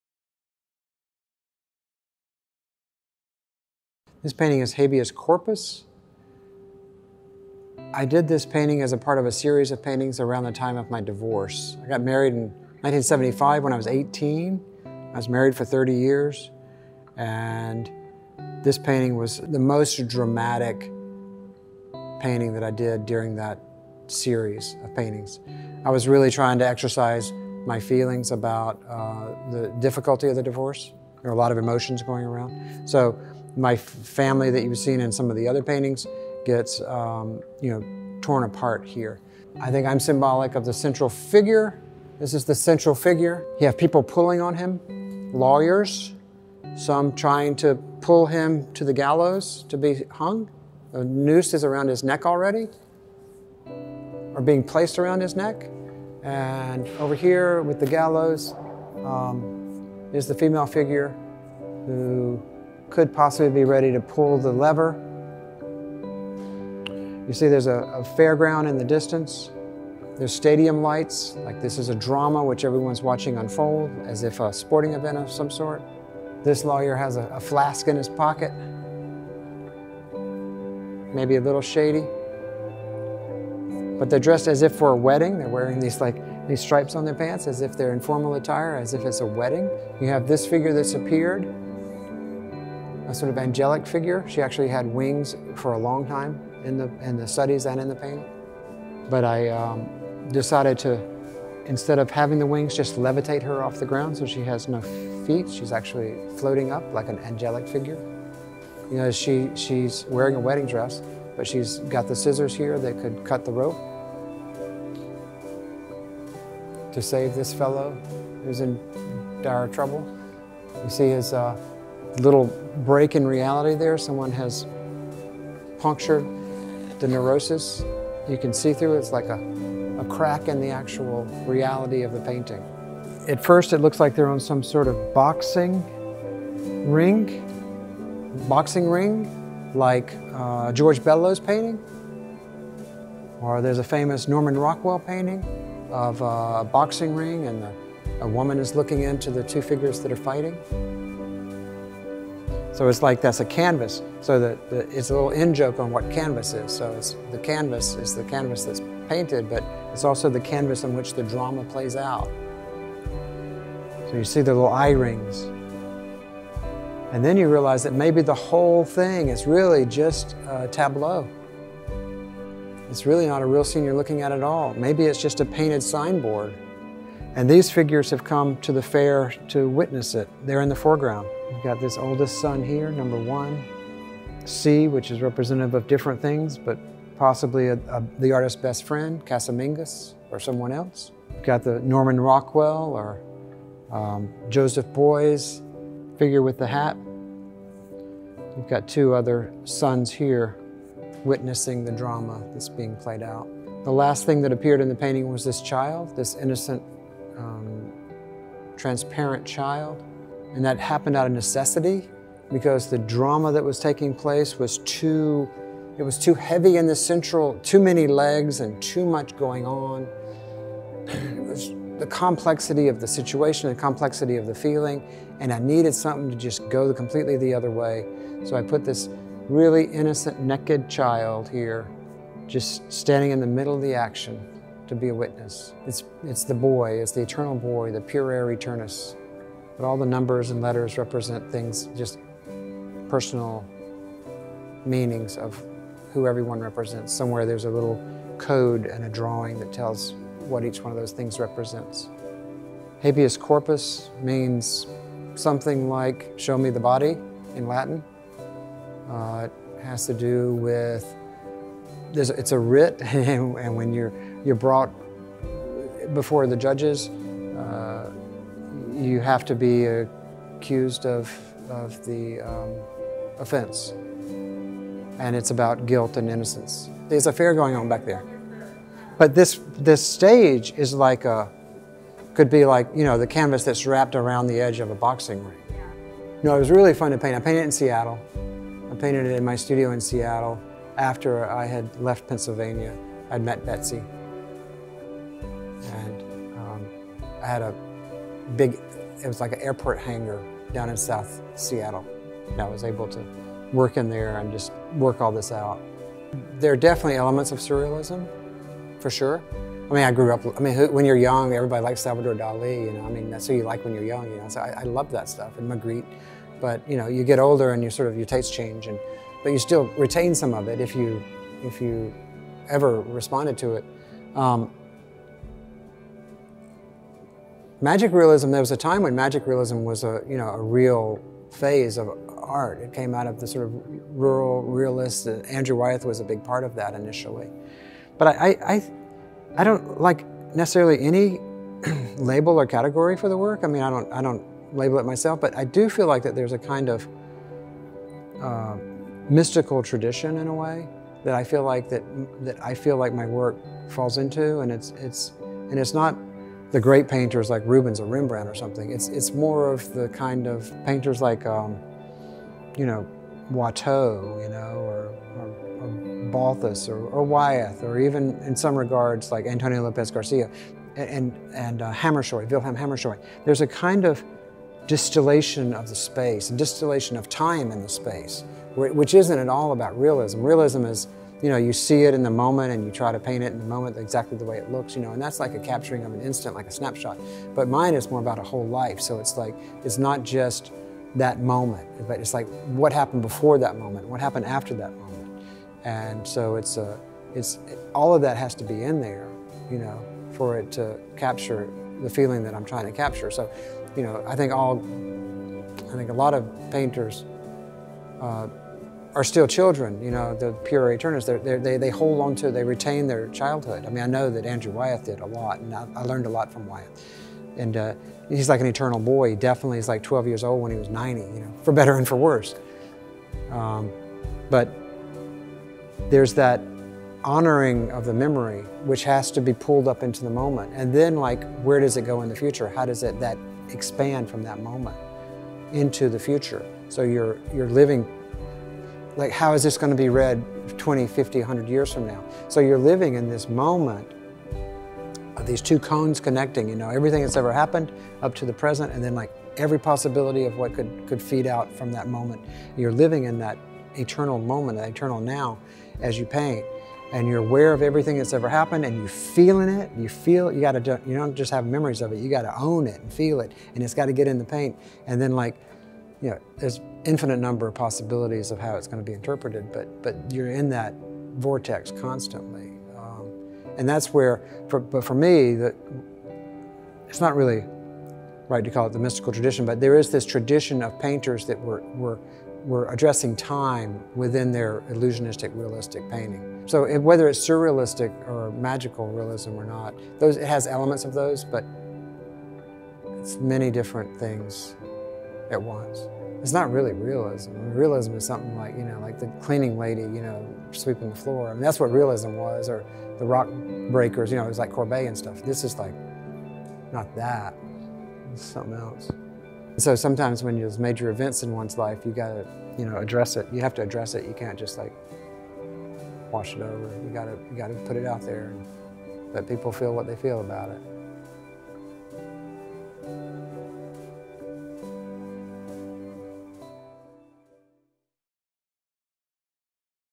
Habeas Corpus with Bo Bartlett
Watch as Bo Bartlett goes into the details of his incredible piece, Habeas Corpus!